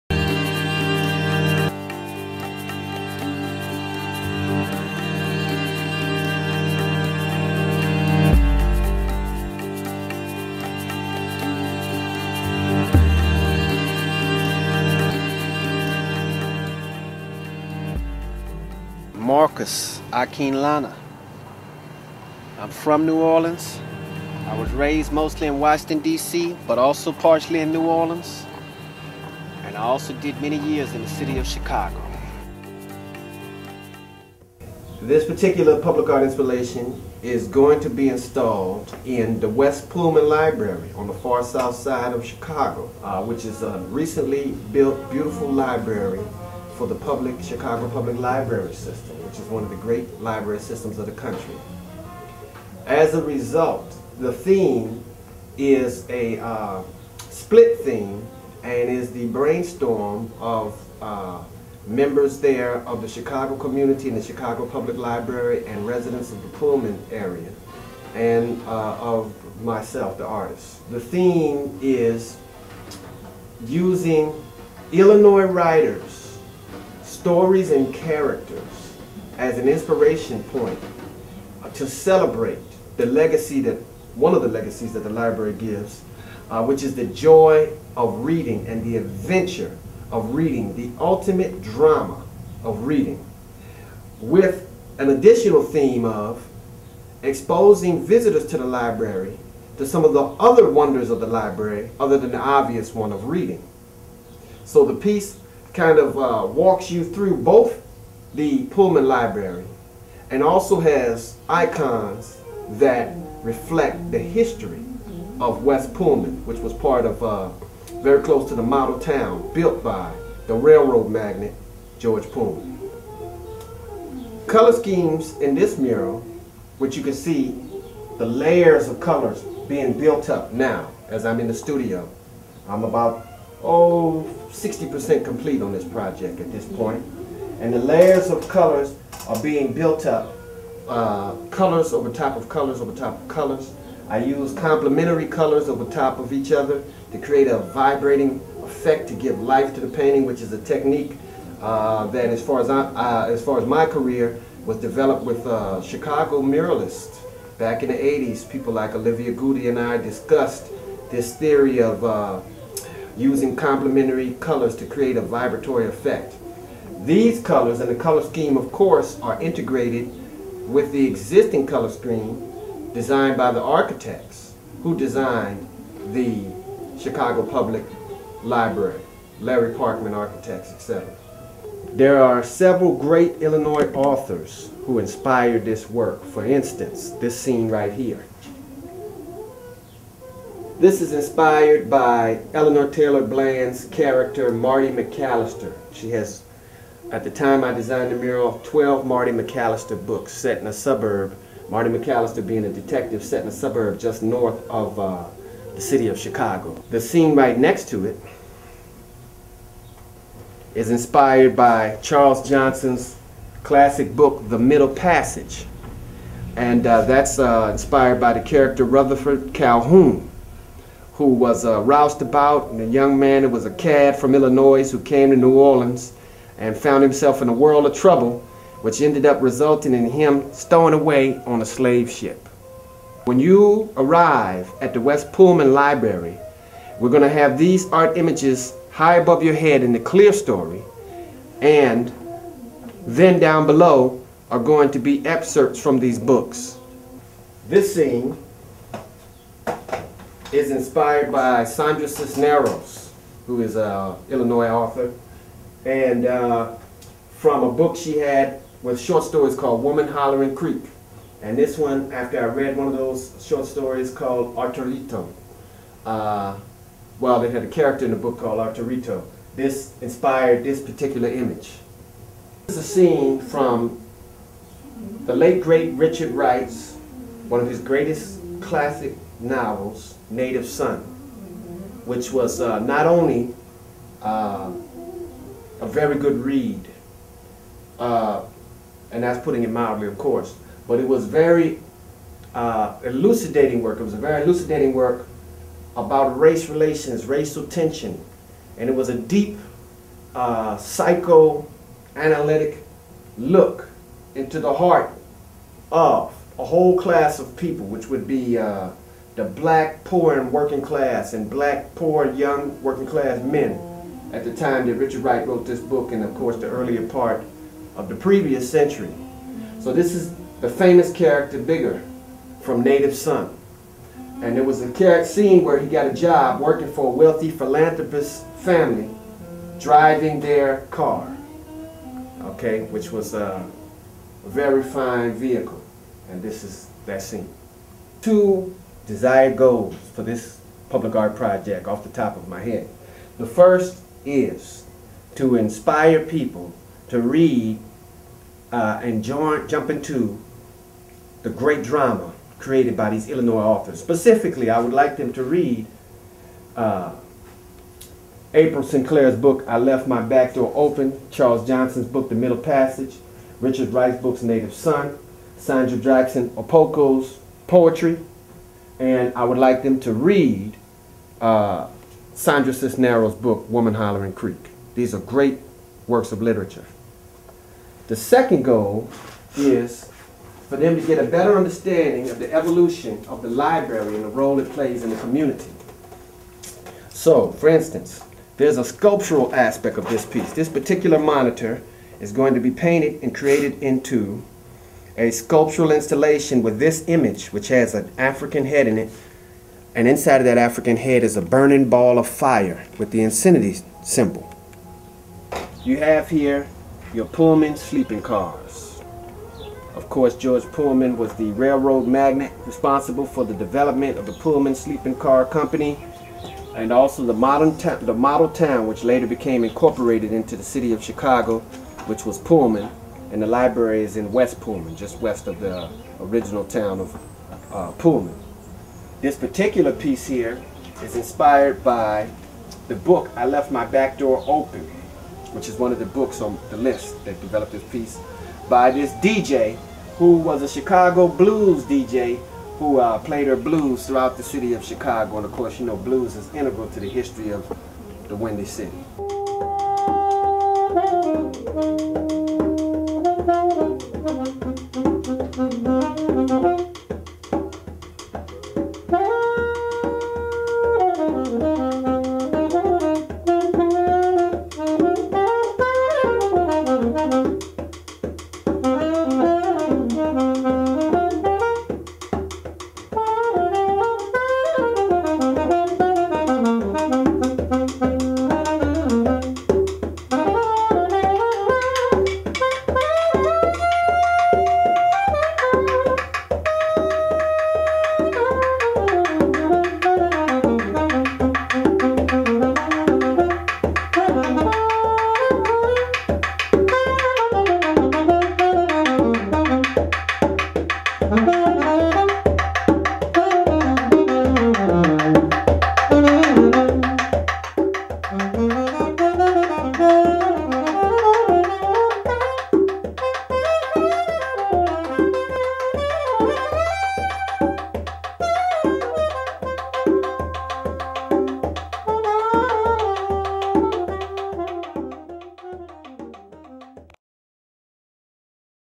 Storytellers Coup D'etat Centrifugal Pullmanesque
Chicago art installation by New Orleans mural artist Marus Akinlana. Exploring and encouraging the joys and wonders of reading, Akinlana creates visual vignettes about the  contributions of native Illinois writers. This piece can be seen in the West Pullman Library in Chicago.